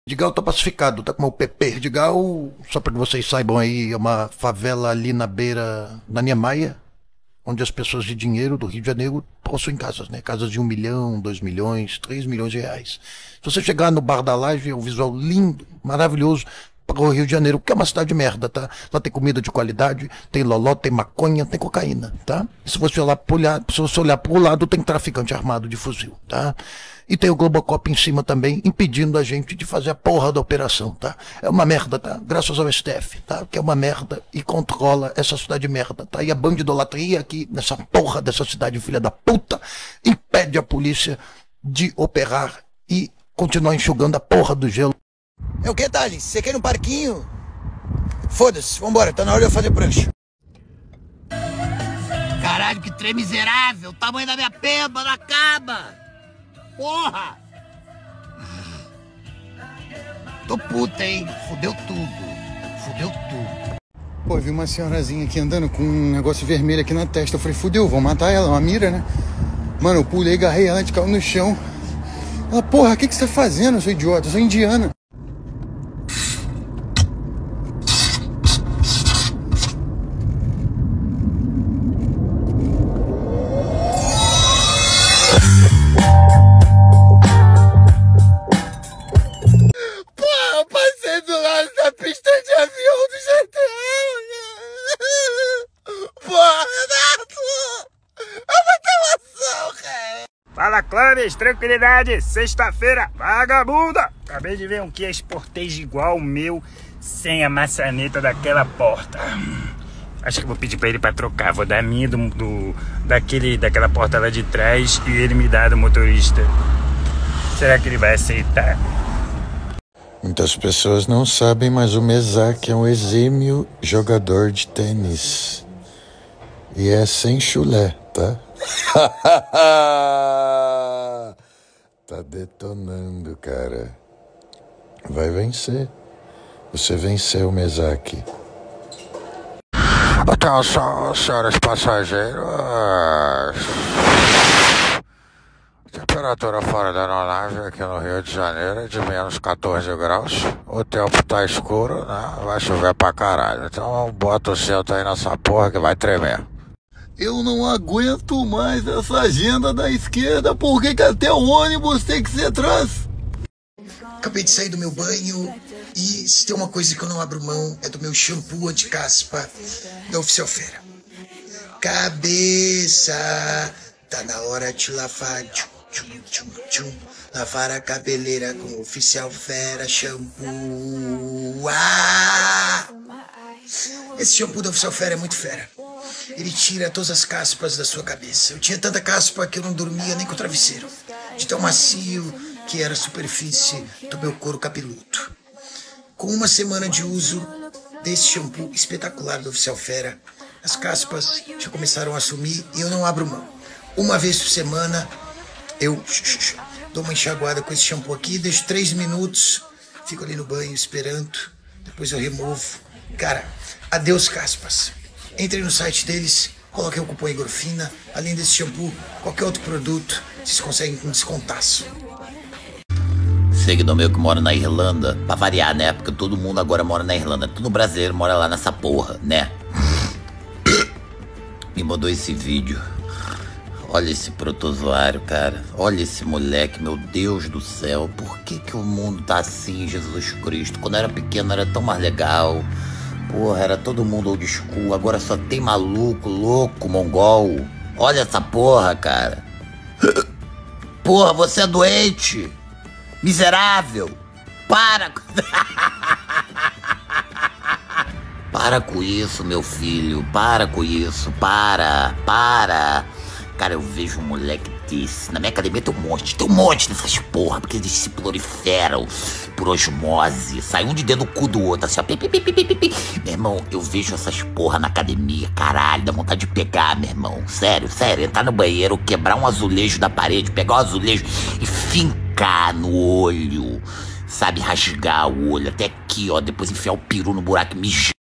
COMPILADO DE STORIES DO IGORFINA PRA RIR MUITO
complicado de imitações do IGORFINAMILGRAU pra chorar de rir